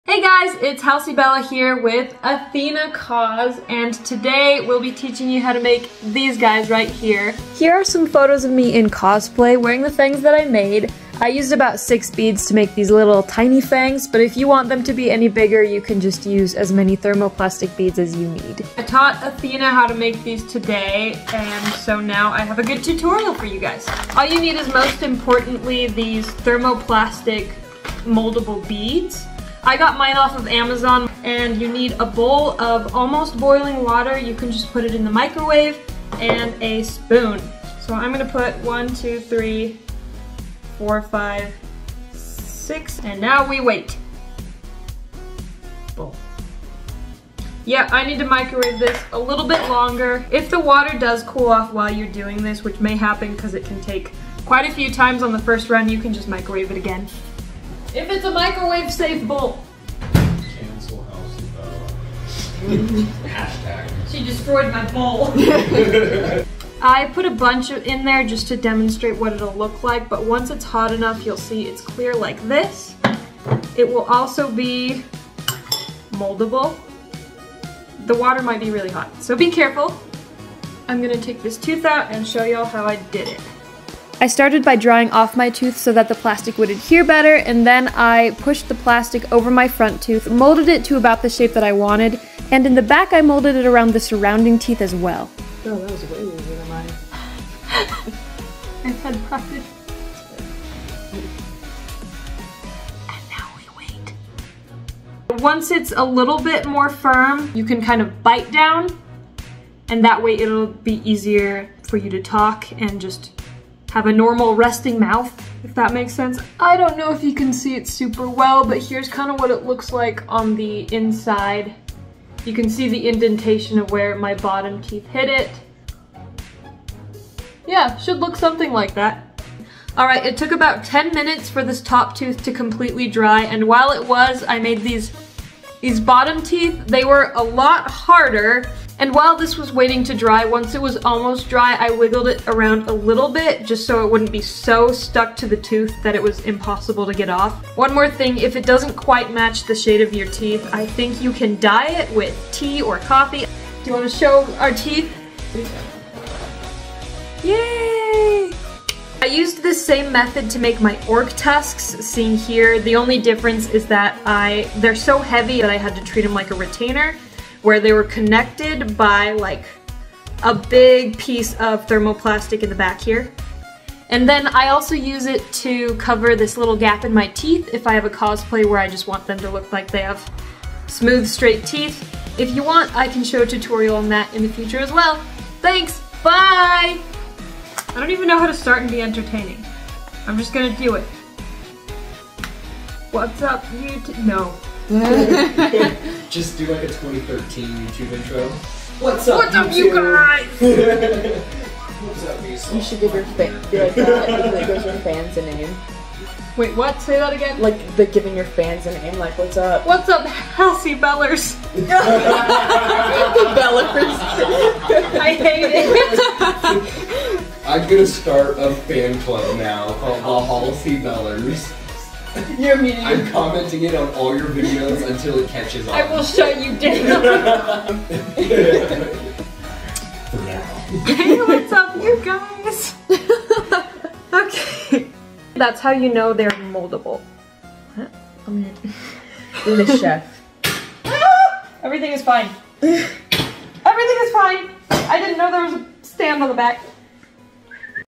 How to Make Fangs for Cosplay and Halloween!
00:00 - Introduction
00:23 - Materials
01:23 - Tips and Tricks
02:23 - Finished Fangs
03:07 - Outtakes and Bloopers
Thermoplastic Beads
I hope you enjoyed the video, and I hope I remember to film more of these in the future lol. Cheers!